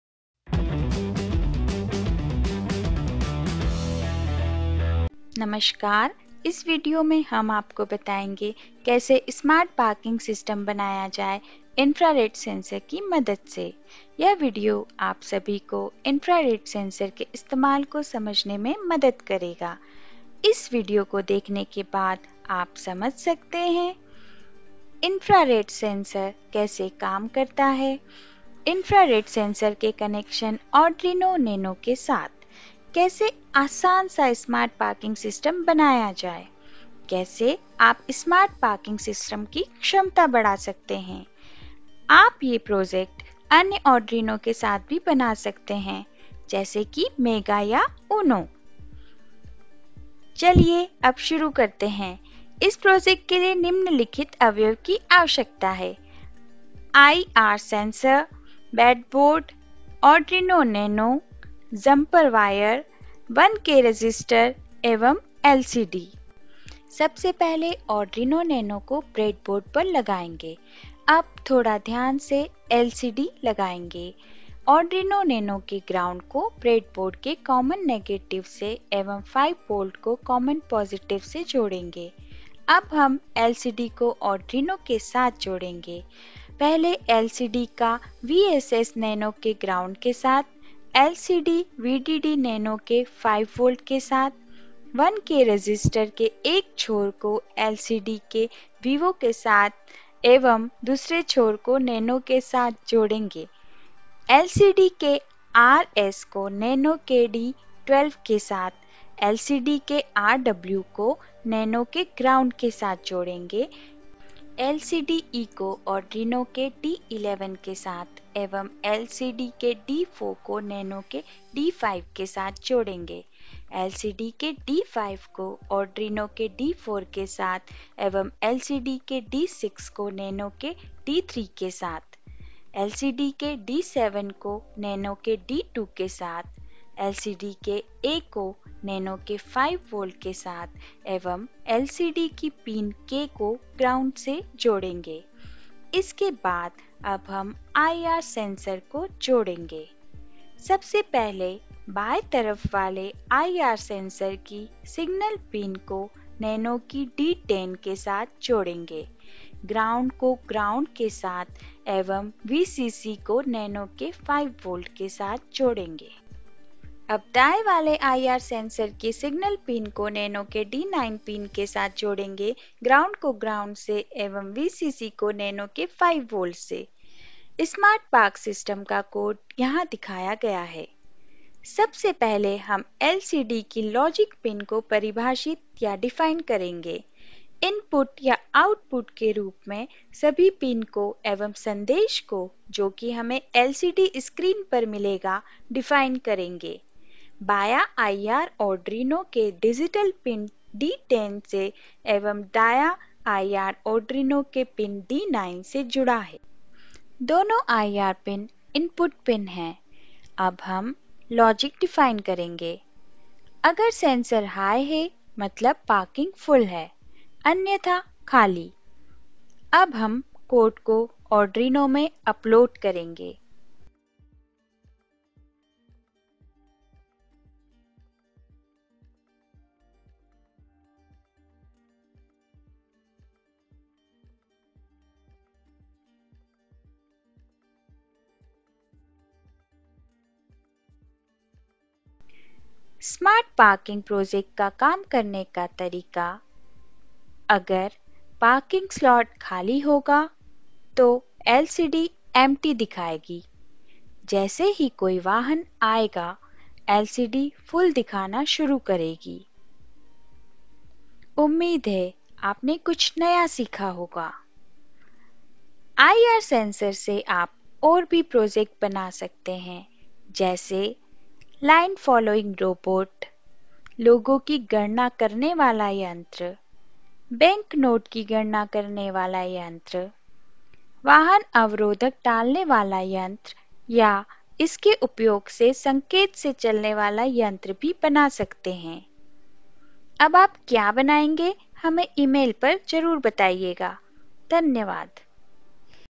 How to Make Smart Parking system in Hindi
This is Hindi version of Smart Parking Sensor, this is simple explanation which will help you to build your own parking system at home.

कैसे स्मार्ट पार्किंग सिस्टम बनाया जाए इन्फ़्रारेड सेन्सर की मदद से ।
यह विडीओ आप सभी को इन्फ़्रारेड सेन्सर के इस्तेमाल को समझने मैं मदद करेगा ।
आई॰आर॰ सेन्सर से आप और भी प्रोजेक्ट बना सकते है जैसे।
लाइन फ़ालोइंग रोबोट।
लोगों की गणना करने वाला यंत्र।
बैंक नोट की गणना करने वाला यंत्र।
वाहन अवरोधक टालने वाला यंत्र या
इसके उपयोग से संकेत से चलने वाला यंत्र भी बना सकते है।